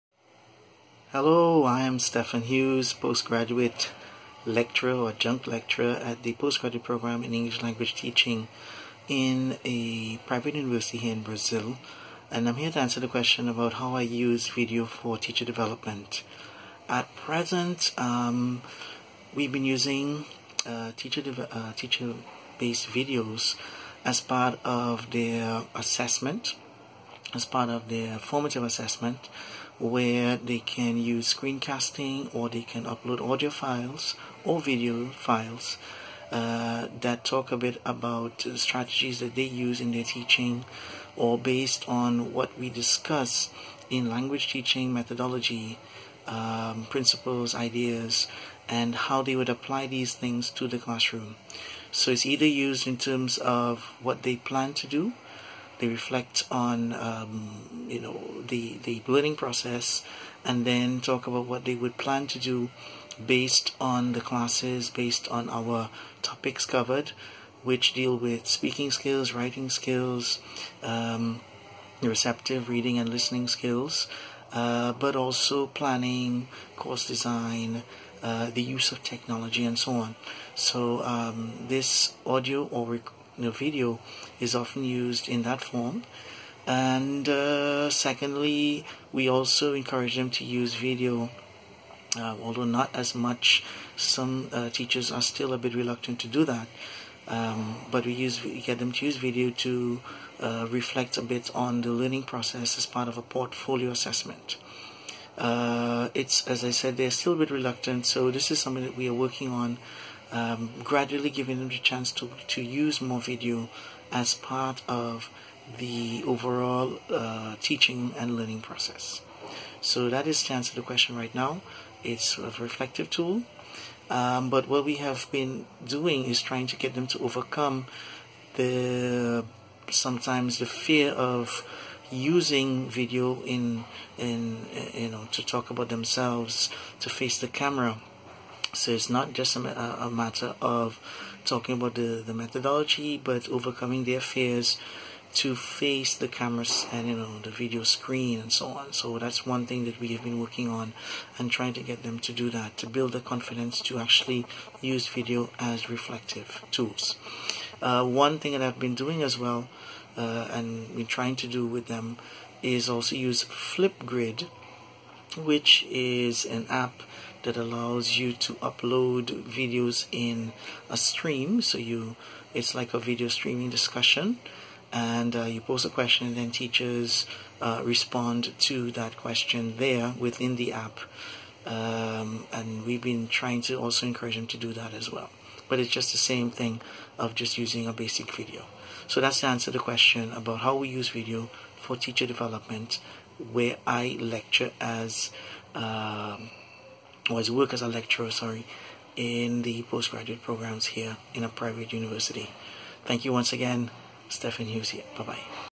How do you use video for teacher development?
An overview of what I'm currently doing with video and audio recordings as an integral part of formative assessment and self reflection.